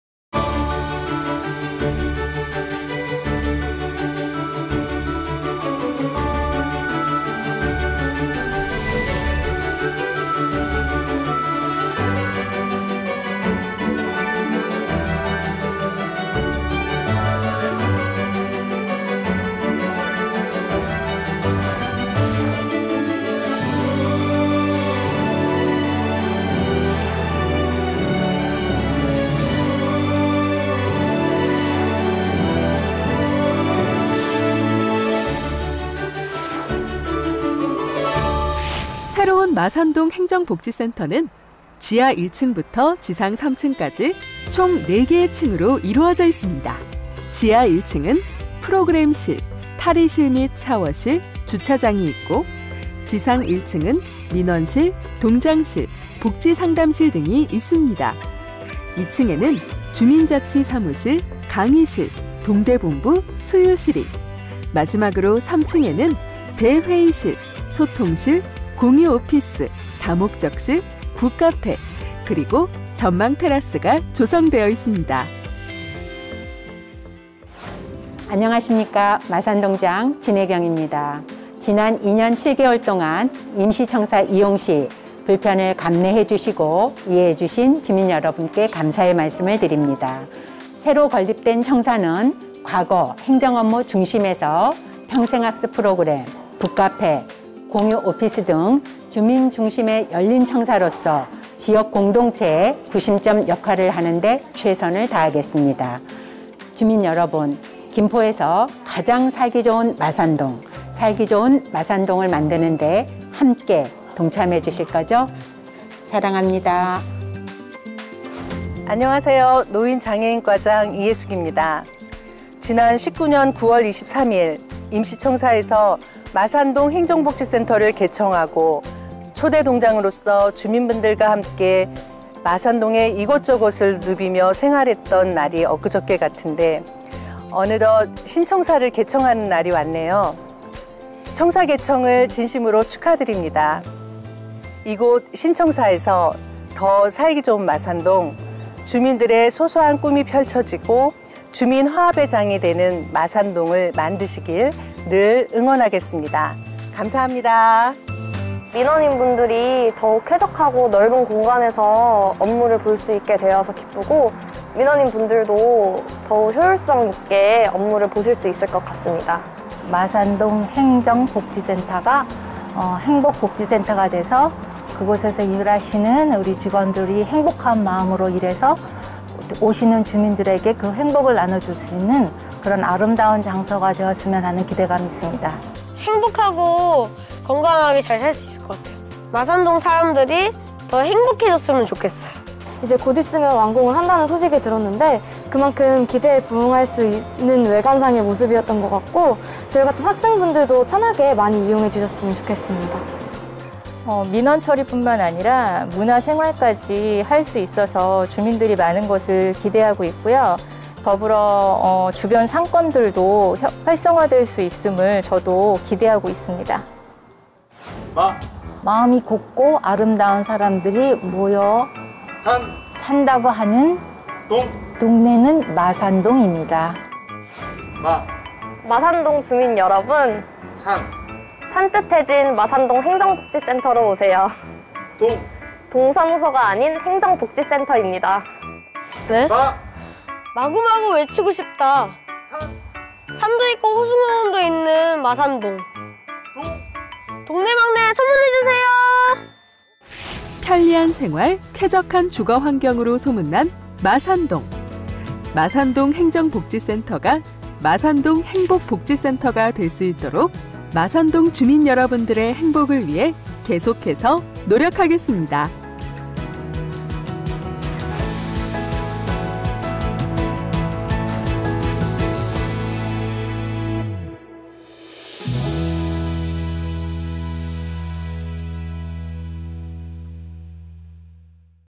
🎉축개청🎉 마산동 행정복지센터가 새롭게 시작합니다❗
높은 산을 뜻하는 ‘마리뫼’에서 변천한 ‘마리미’로 불리웠다는 마산동

2019년 9월 23일 개청되었던 마산동 행정복지센터가
2022년 5월 2일, 신청사로 이전하여 업무를 개시했습니다!🎉

이제 마산동 행정복지센터는 새로운 곳에서 새롭게 시작하려 합니다
마산동 행정복지센터가 행복복지센터가 될 수 있도록 마산동 주민 여러분들을 위해 계속해서 노력해 나가겠습니다🤗